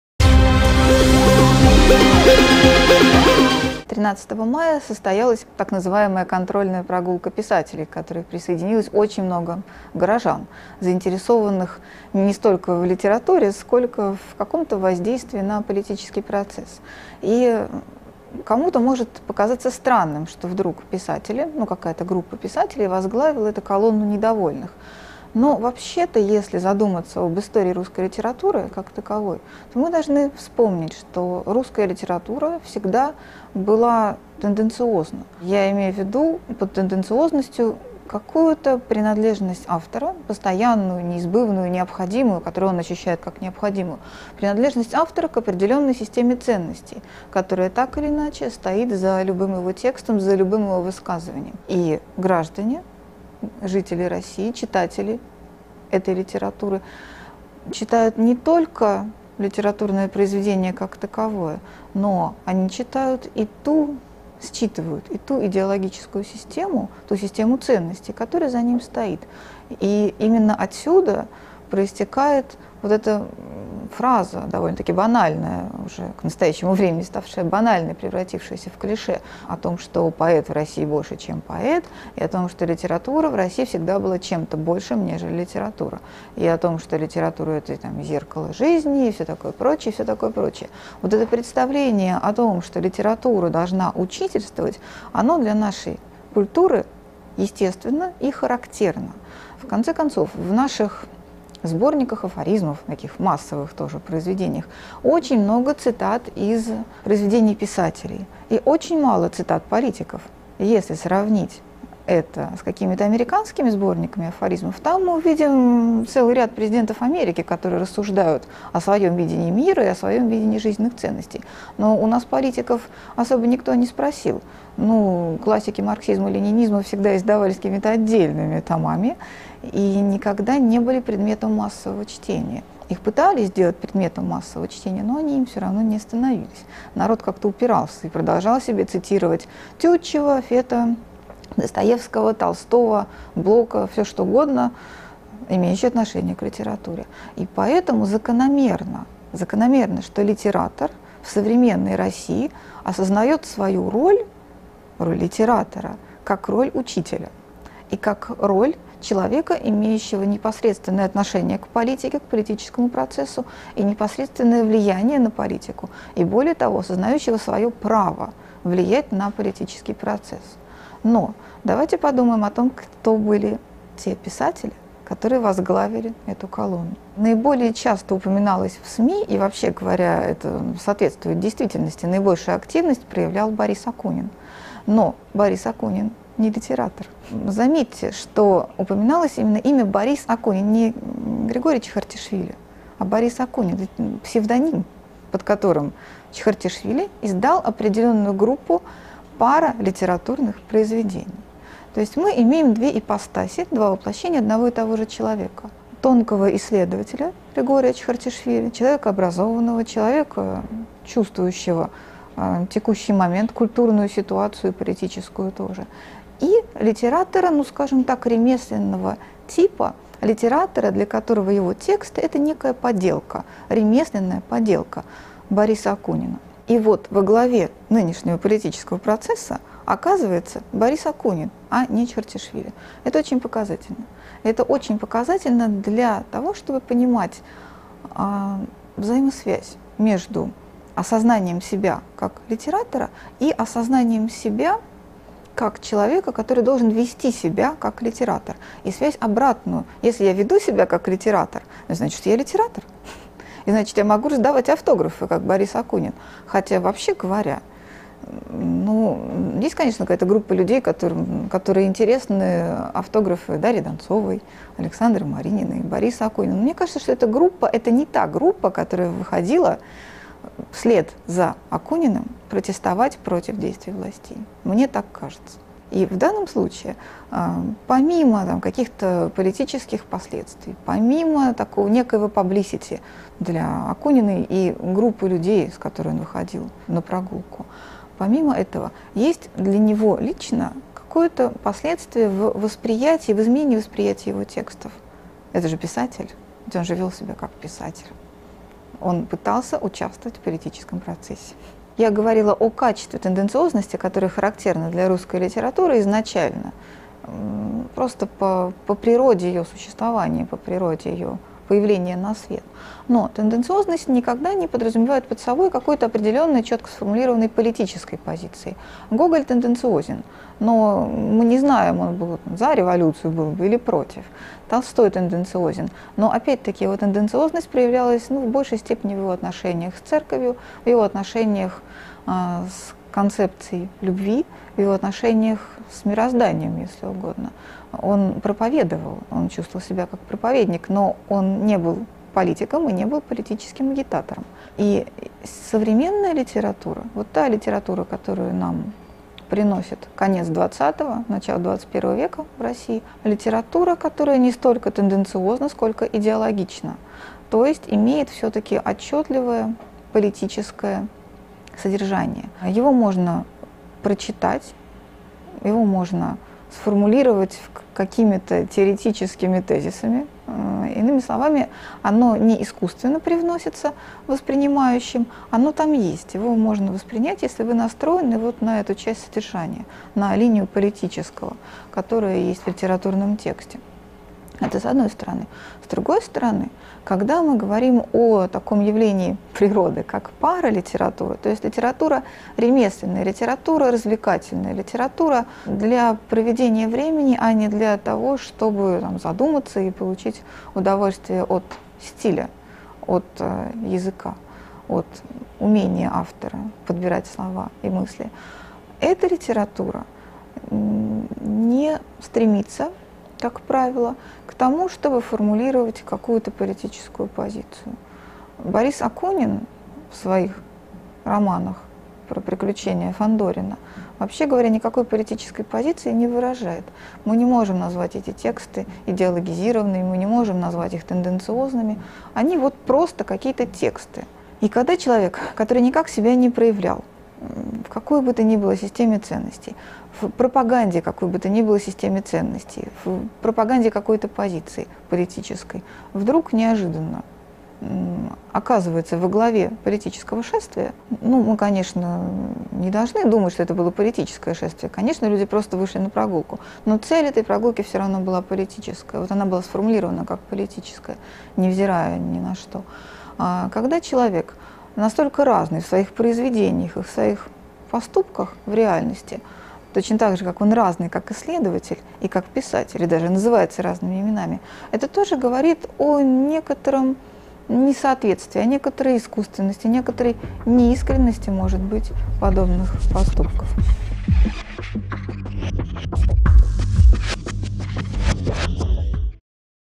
Анна Потсар - Пара-литераторы на марше
Кто и почему возглавил прогулку писателей по бульварам? Какая идеология присутствует у этих манифестантов? Мнение филолога Анны Потсар.